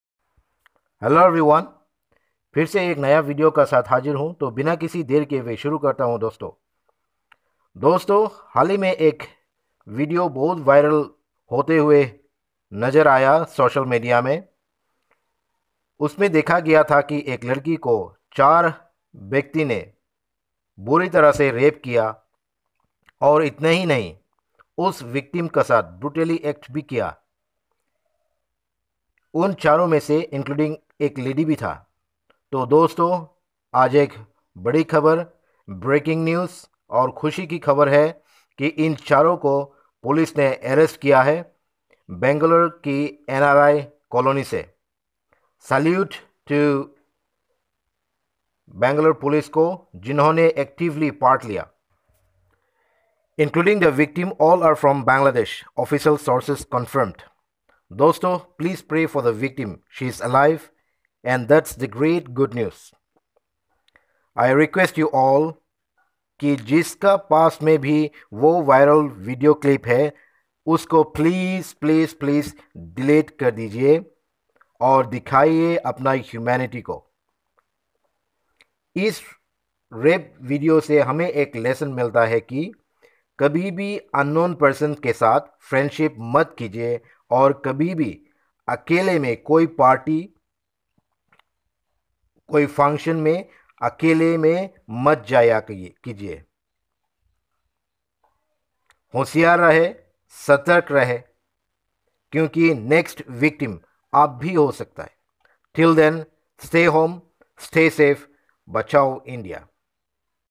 Rapist Arrested in Bengaluru
All the rapists are arrested by Bengaluru Police from NRI Colony. And all were from Bangladesh.

Photo Credit:
tc9kannada
eastmojo
indiatoday
the hindu
outlook
ndtv